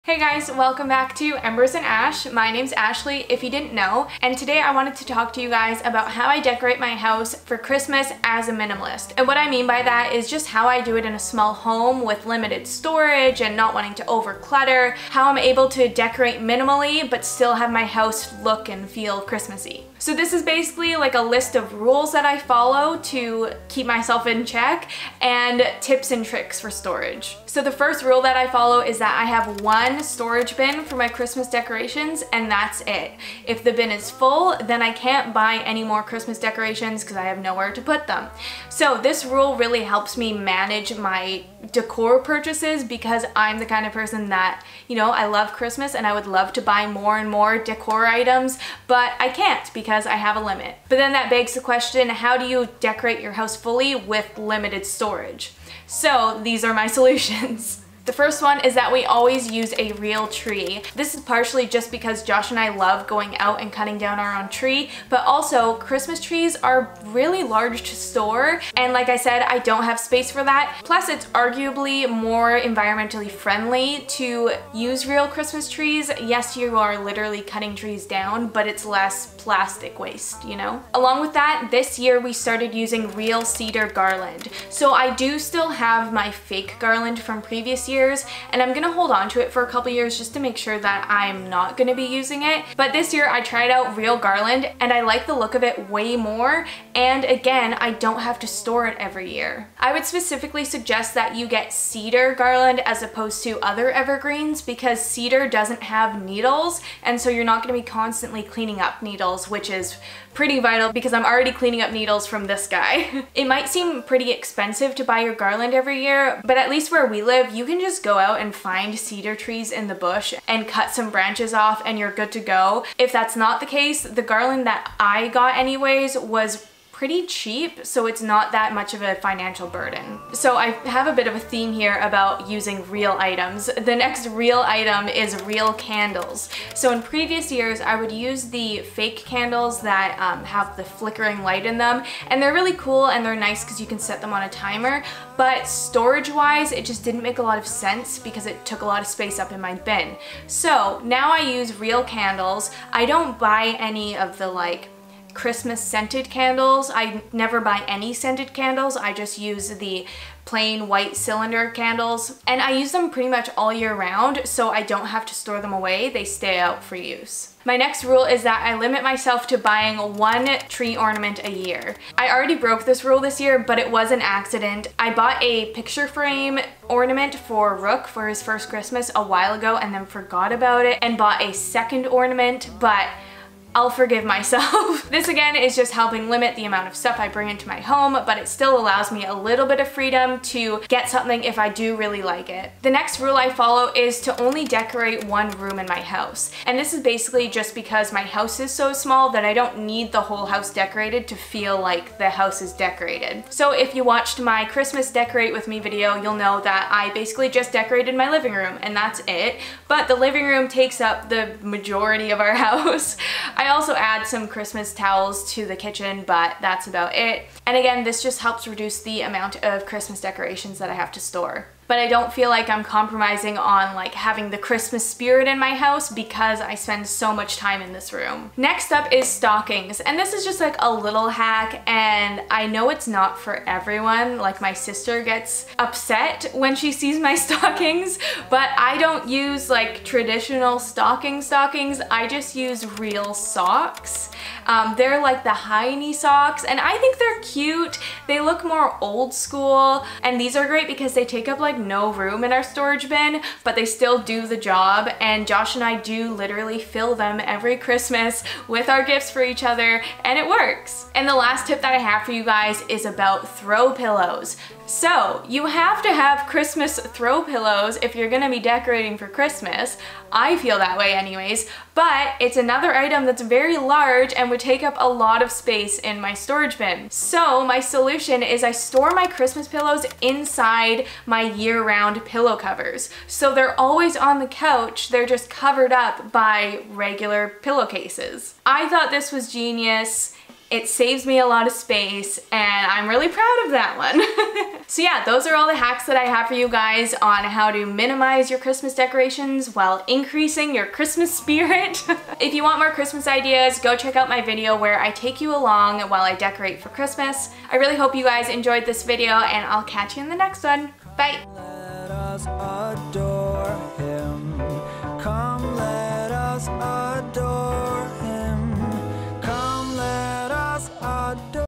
CHRISTMAS STORAGE HACKS & SOLUTIONS
HELLO
In todays video I tell you all my tips and tricks on how I minimize Christmas decorations while maximizing the Christmas spirit! I do this my limiting my storage in smart and intentional ways. I hope you stick around and learn some storage tips and hacks!

Scentless Dripless Candles
Fresh Air Memories Ornament
Red Stockings
Christmas Pillows

MailBox
BC, V3Y 2G7
Canada